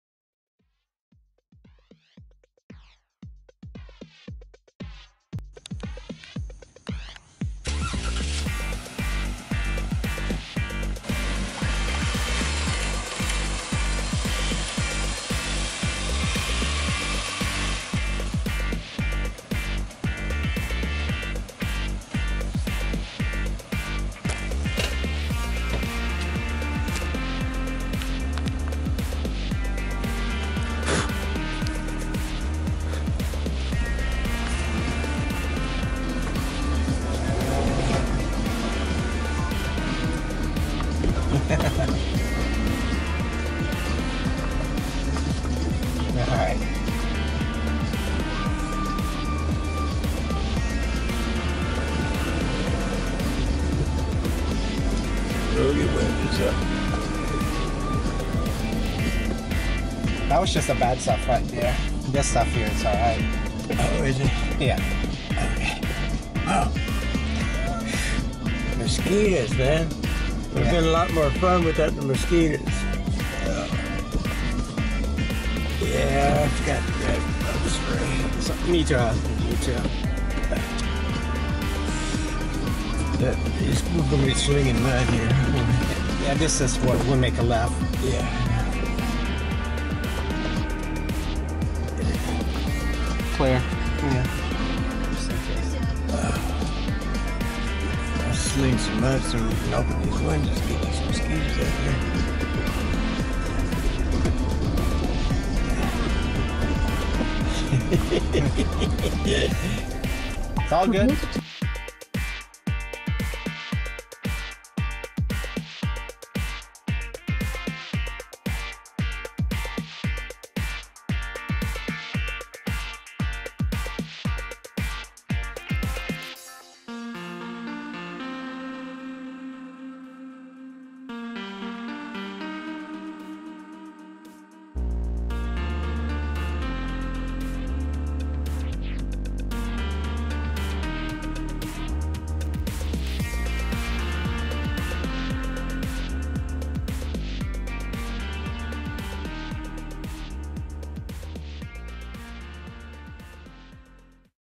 Twelve Mile Swamp Conserve Area
Richard F and I went on a short trip this morning and were able to get stuck (Jeeps first need of its winch). All and all, great day in our FL swamps.